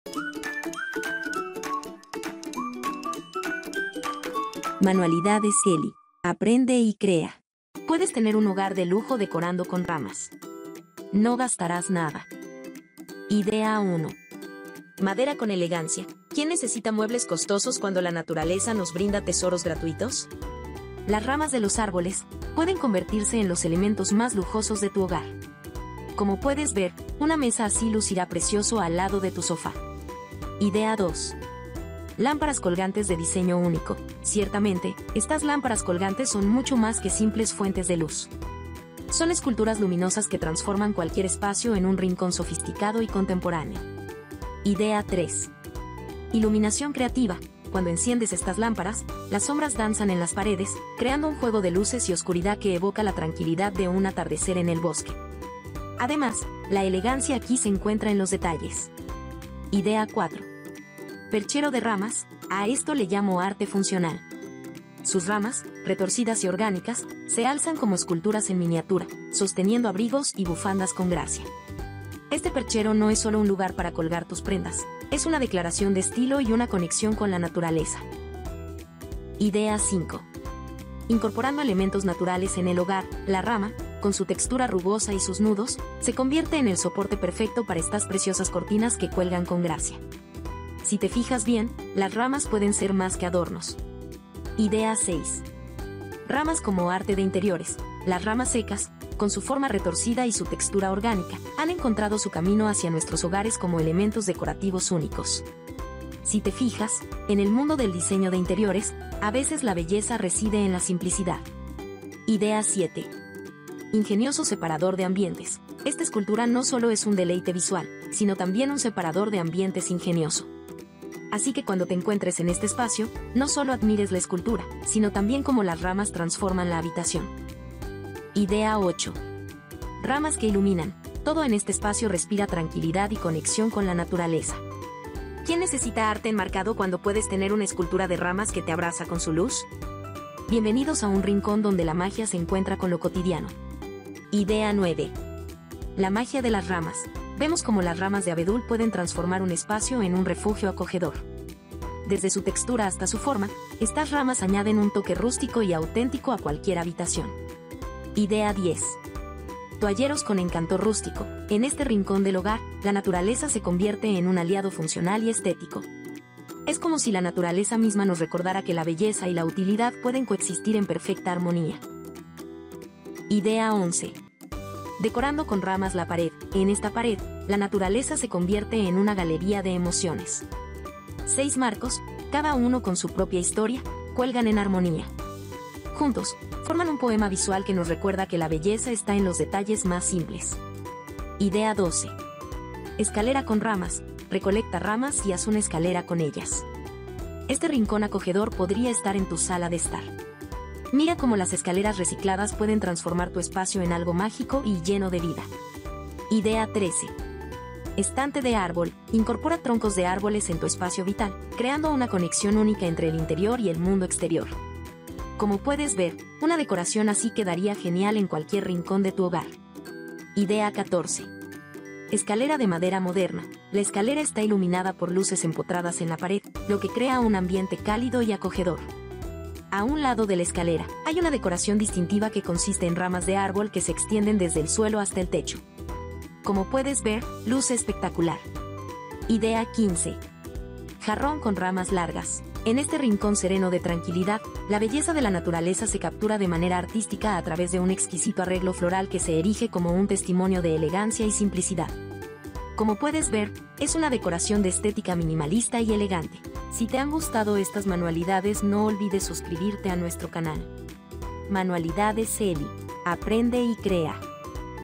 PUEDES TENER UN HOGAR DE LUJO DECORANDO CON RAMAS ¡NO GASTARÁS NADA!
Sabías que puedes tener un hogar de lujo decorando con ramas?. ¡No gastarás nada! y además esas ramitas que encuentras en el parque o en tu propio jardín pueden transformar tu hogar en un espacio sofisticado y lleno de estilo.

INICIO   | 0:00
IDEA   1 | 0:13
IDEA   2 | 0:30
IDEA   3 | 0:44
IDEA   4 | 0:59
IDEA   5 | 1:19
IDEA   6 | 1:34
IDEA   7 | 1:52
IDEA   8 | 2:08
IDEA   9 | 2:24
IDEA 10 | 2:38
IDEA 11 | 2:53
IDEA 12 | 3:11
IDEA 13 | 3:26
IDEA 14 | 3:40
IDEA 15 | 3:59